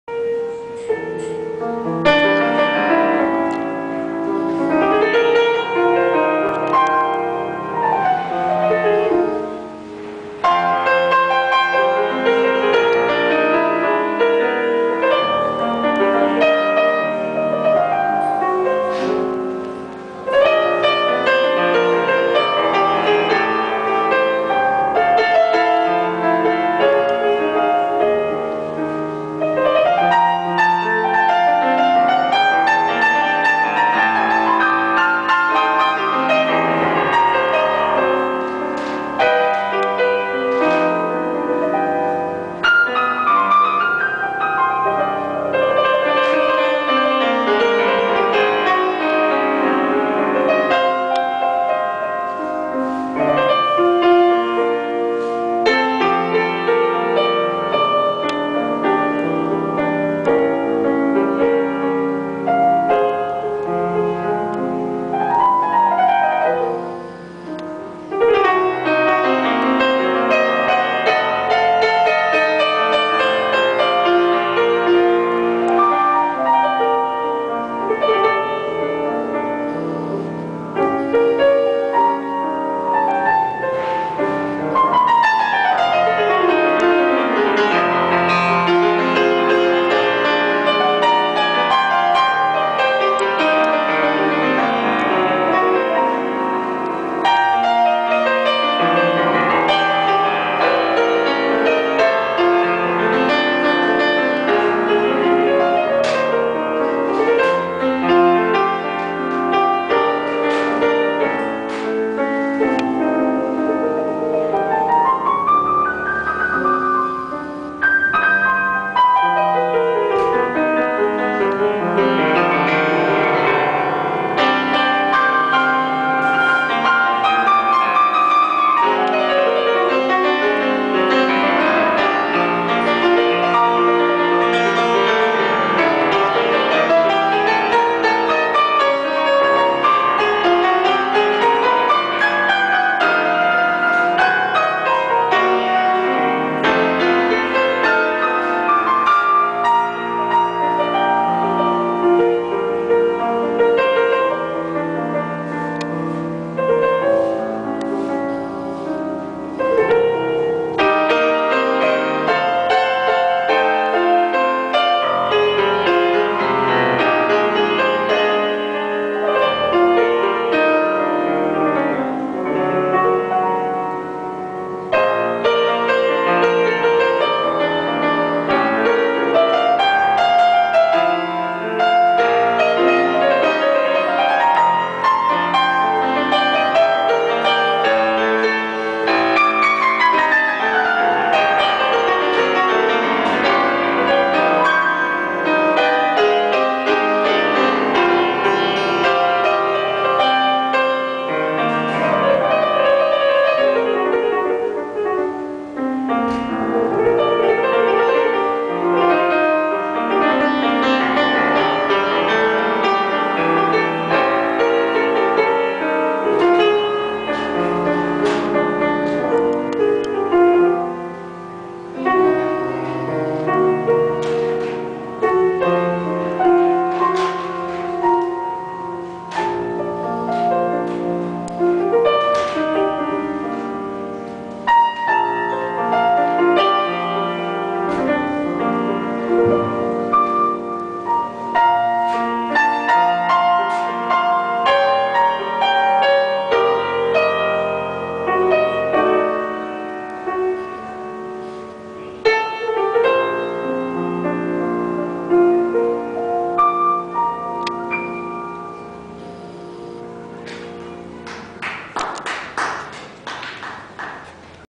Piano's Maene 1
Solo jazz  optreden voor beurs Piano's Maene in Antwerpen door pianist Mike Julien